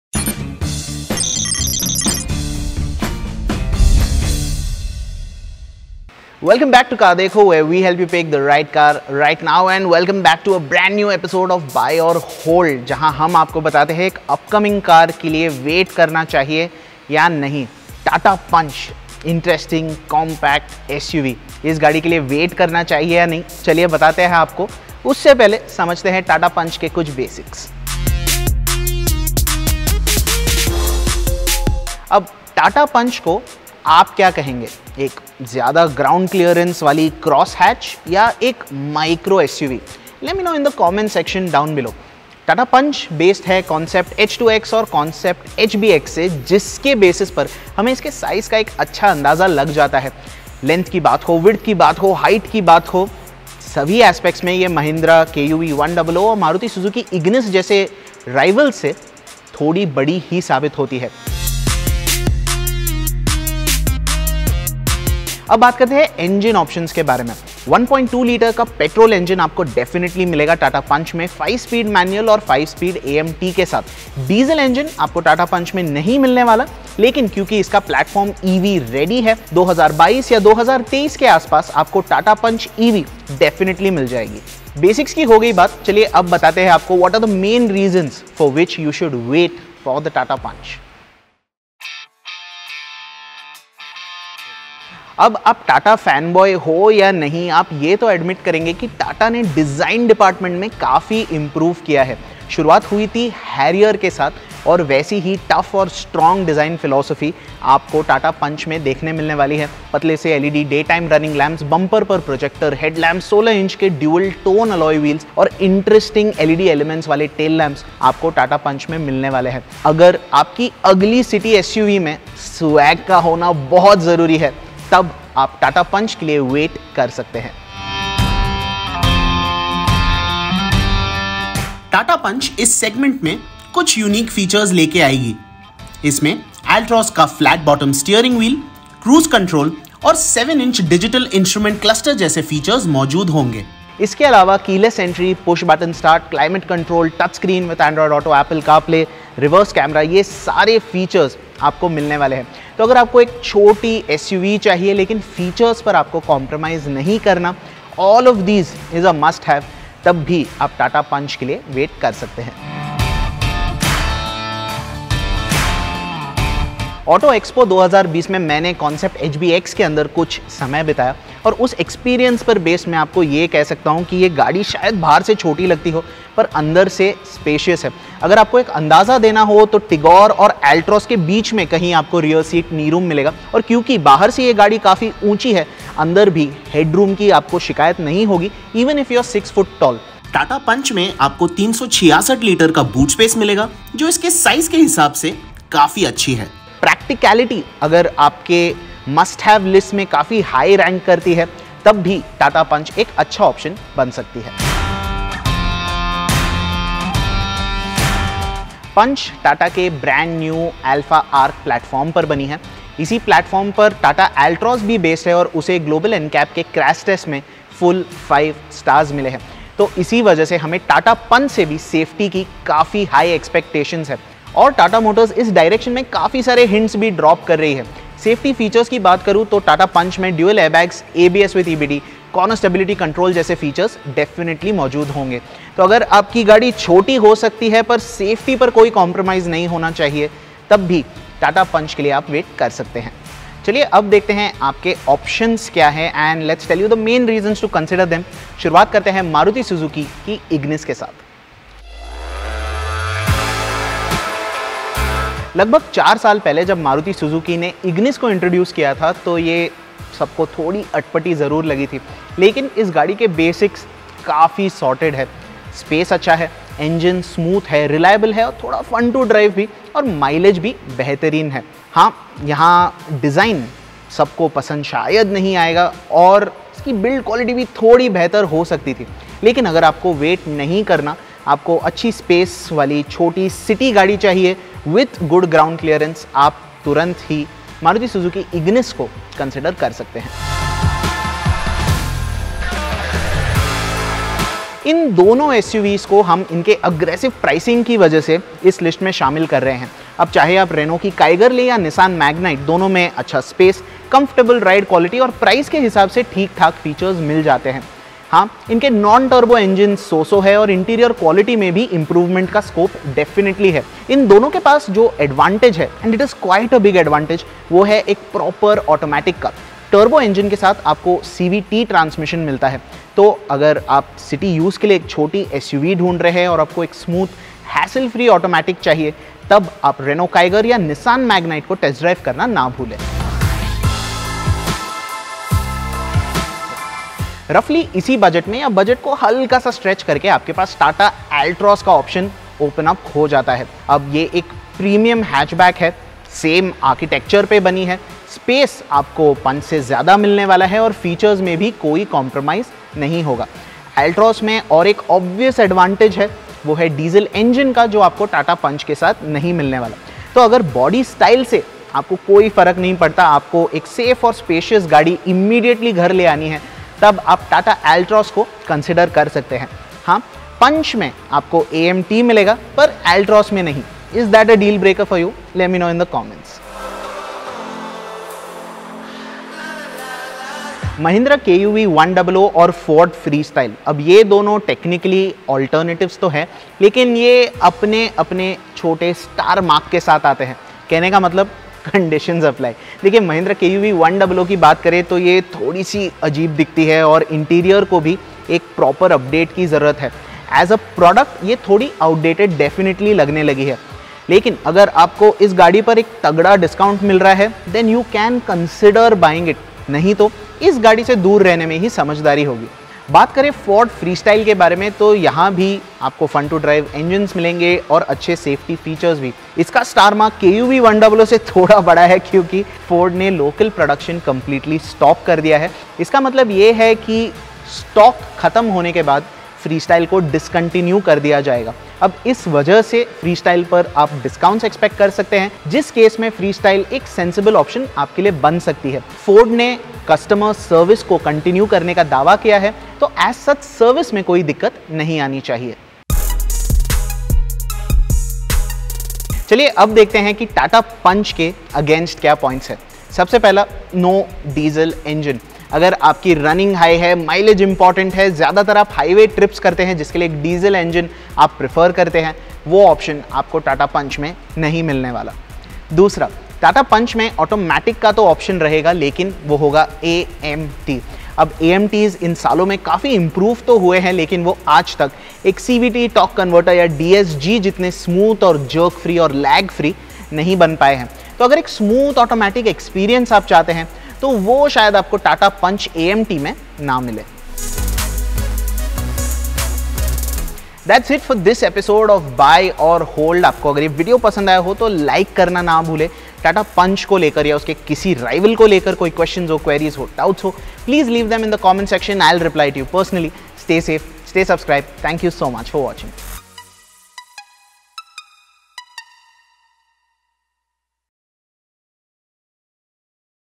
Tata Punch vs Magnite, Kiger, Ignis, KUV100 and More! | WAIT करो, समझदार बनो!
In this video, we discuss the positives and negatives of the Tata Punch and tell you if it’s worth waiting for. The Tata Punch scales down the compact SUV formula while offering the ease of use of a hatchback. Should you hold for the Tata Punch or buy from exisitng options?